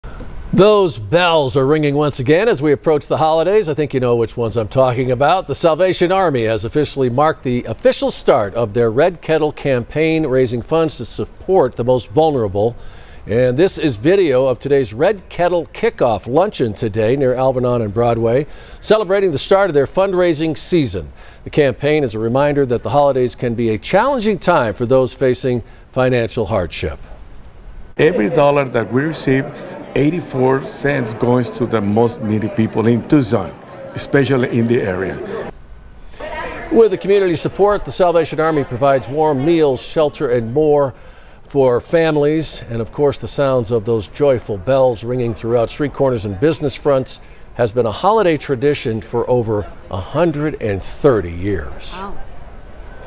Salvation Army holds annual Red Kettle Kickoff Luncheon
This festive luncheon marks the official start of the Army’s Red Kettle Campaign, a longstanding holiday tradition that raises critical funds to support families in need throughout Pima County.